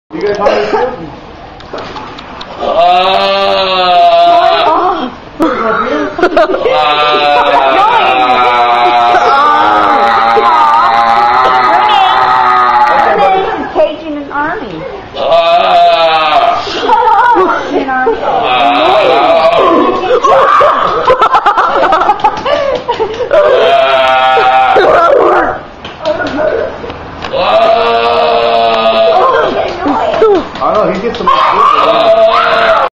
seals gone wld
well heres some more seal time at the beach. the damn seal got all the attention. the other ones were starving out there with a box of hot pockets. damn hoes